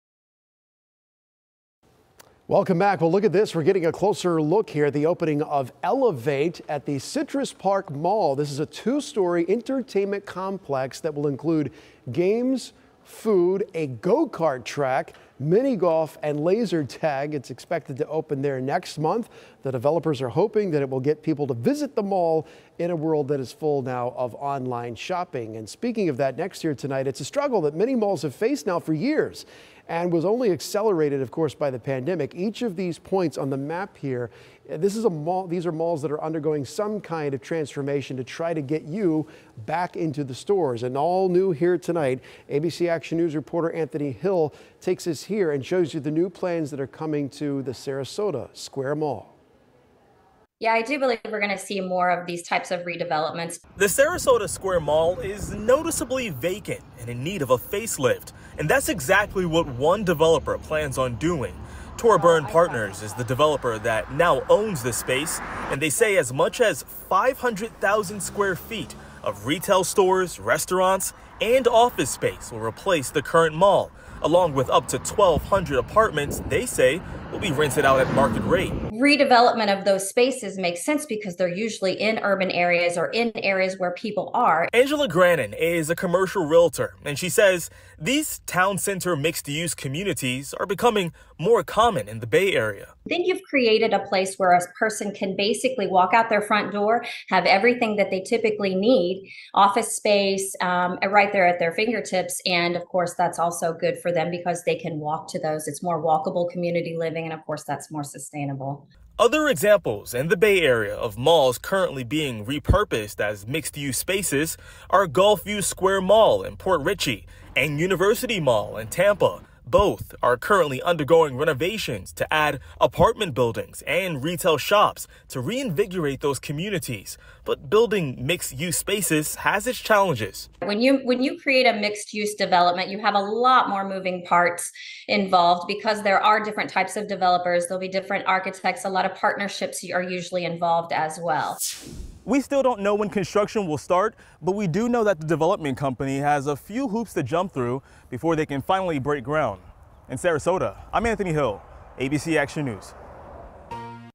Florida developer makes proposal for vacant mall property
Full Story The Sarasota Square Mall is noticeably vacant. Local residents walk up and down its halls daily past empty storefronts, some just cutting through to get to the AMC Theatres, Costco, or J.C Penney while others are just trying to get their steps in.  The majority of the mall is silent and offers a glimpse into what was once a bustling shopping mall.